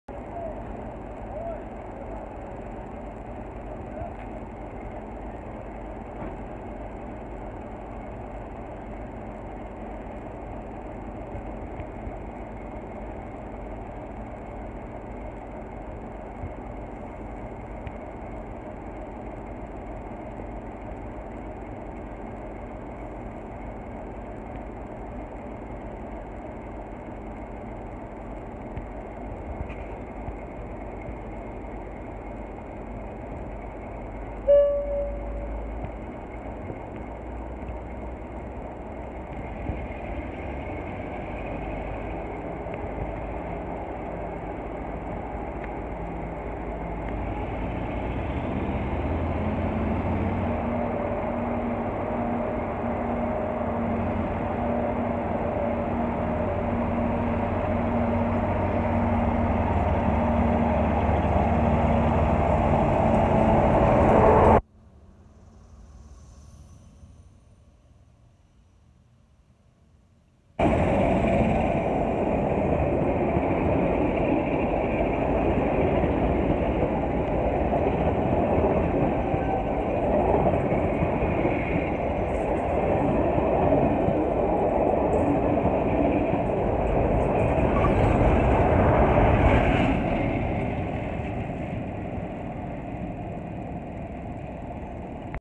WCRC 47746 leads 47854 with a York to Chesterfield shuttle service
West Coast Railways Company class 47746 "Chris Fudge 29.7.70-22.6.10" leading 47854 "Diamond Jubilee" seen departing Sheffield Railway Station working 1Z77 York to Chesterfield Shuttle service  due to Cross-Country being on strike WCRC are giving a helping hand on Sunday 25 of August 2019